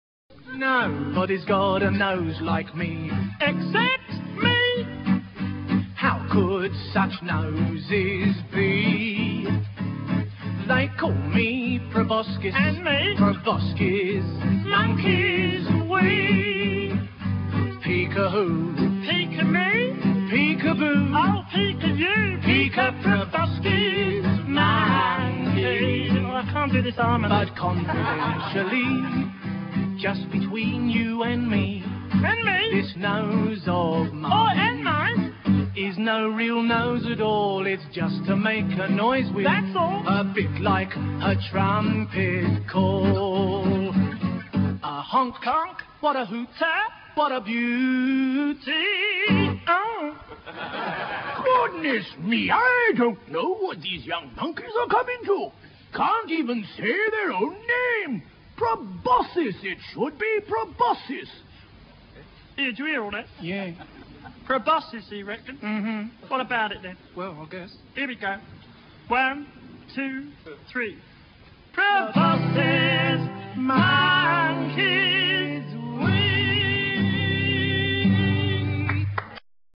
proboscis monkeys song
A nosey monkey froma children's TV show.

What is in a name? Quite a lot if you go around using the wrong one or use the correct one derogatively.